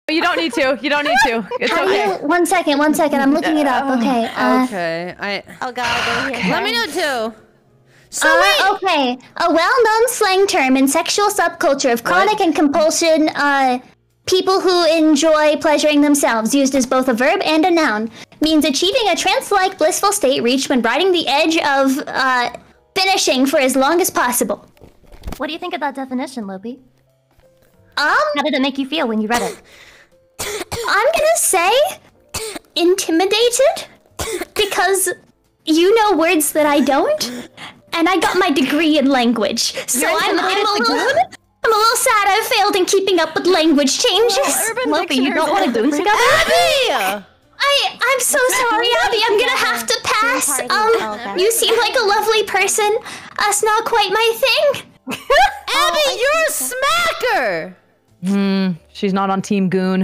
What's a Goon Cave? Filian & Friends
Oh boy. Abi's really done it now. Lopi is reading the definition of gooning out loud, on stream. Demonitization here we come.